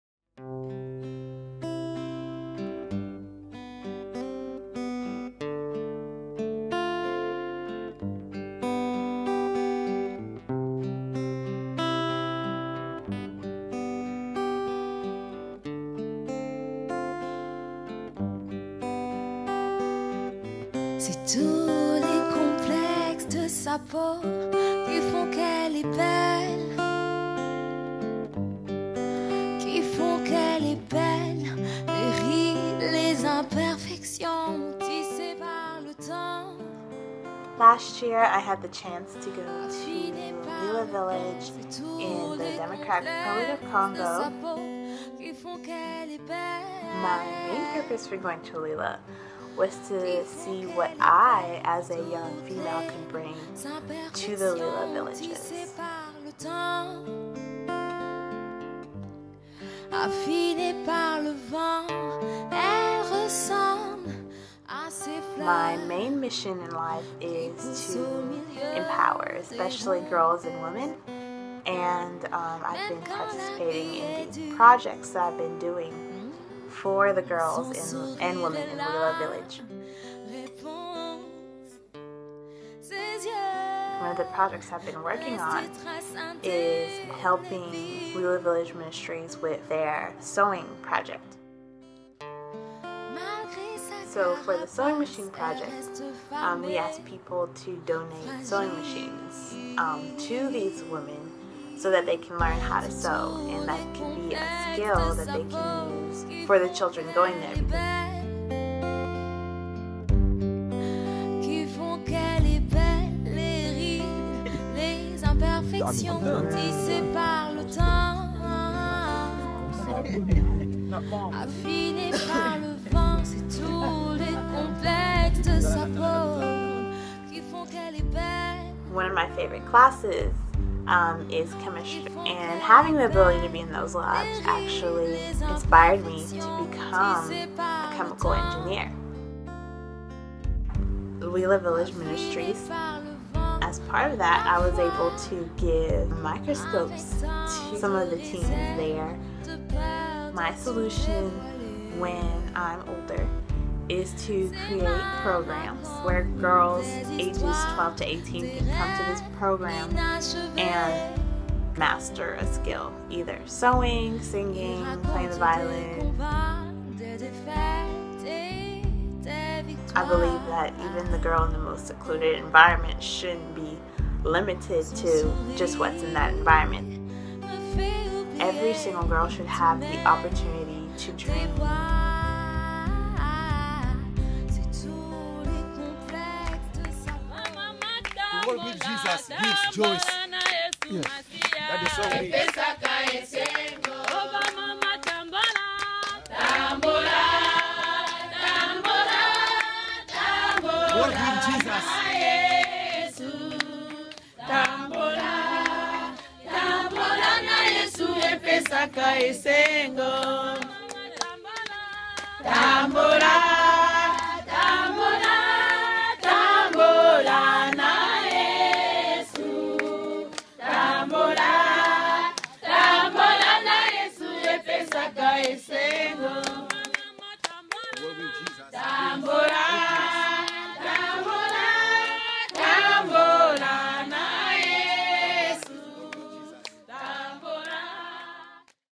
Educate her to Empower her
Hear my personal story and the roots of where my business Esther Nzuzi, came to be.
Esther Nzuzi LLC is a social enterprise aimed to inspire young girls and women across the world to embrace their self-worth. to see how you can make a difference.

Video made by Panach Productions